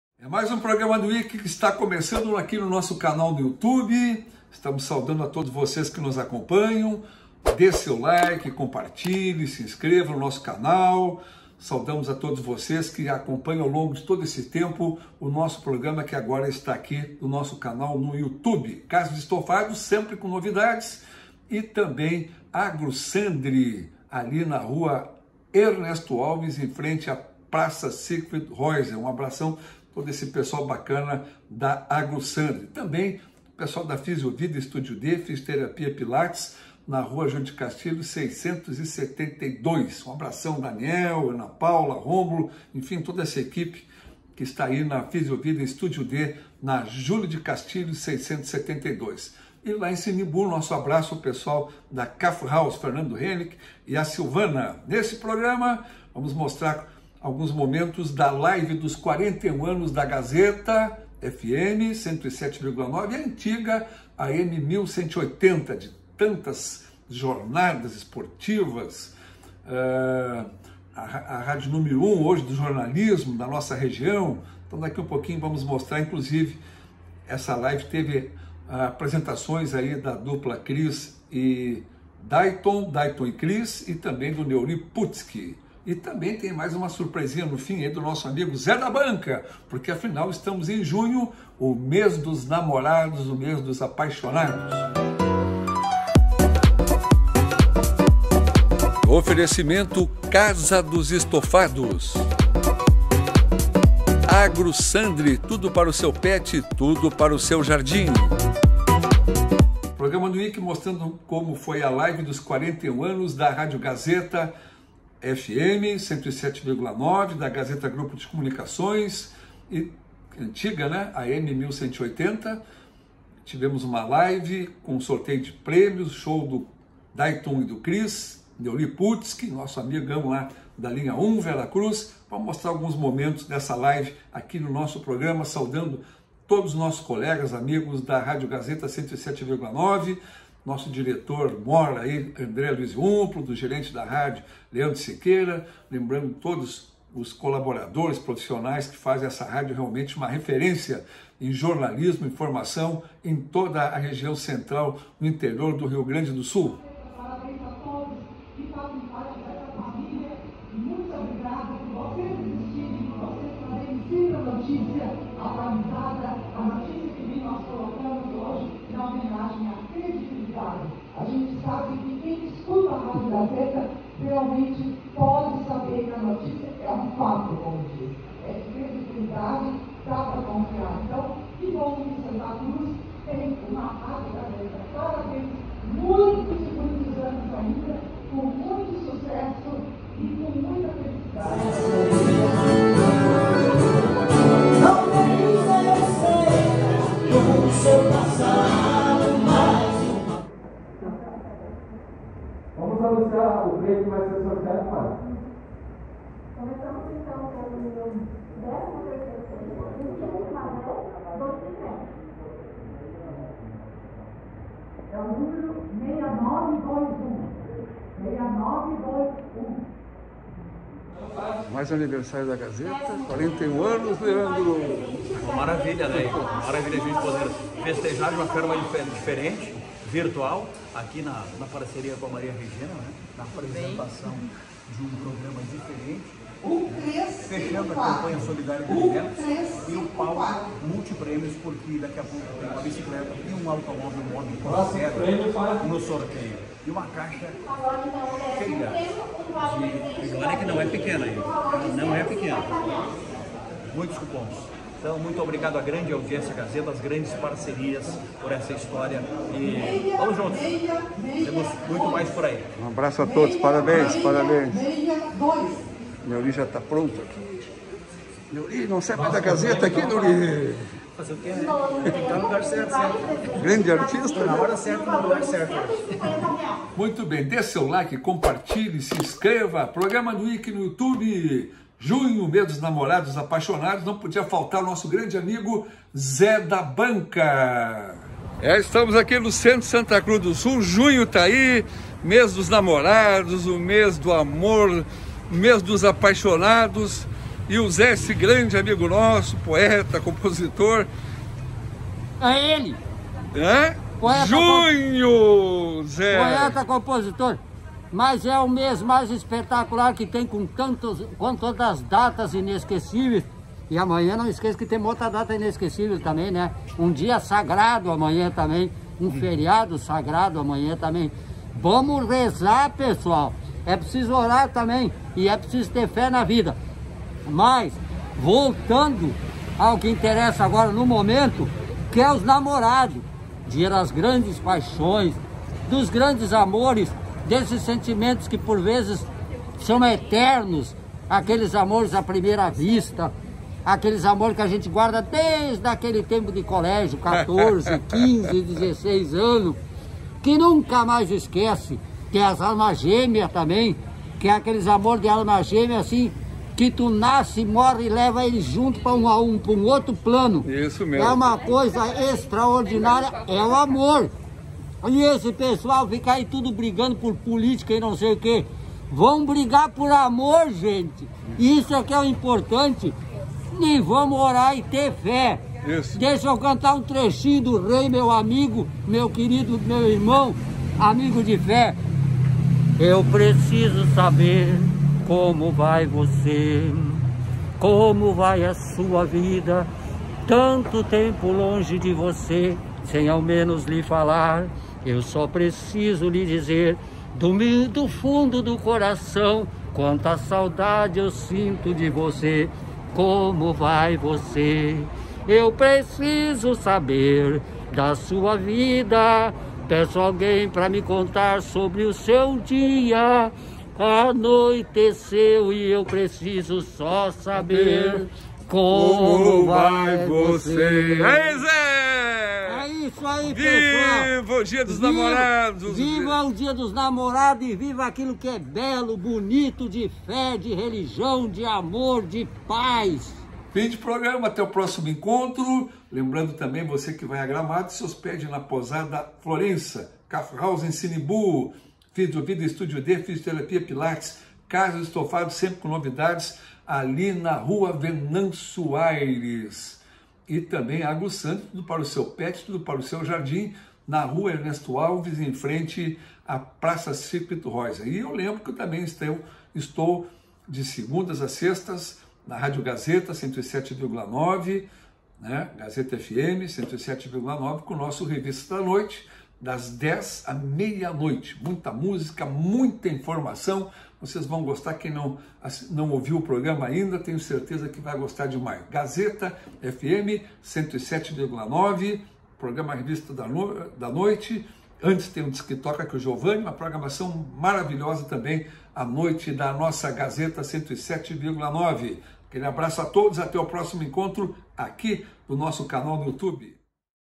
Live Radio Gazeta, Mensagem do Zé da Banca
Um pouquinho da live dos 41 anos da Gazeta 107,9 FM. E uma mensagem muito bacana do Zé da Banca!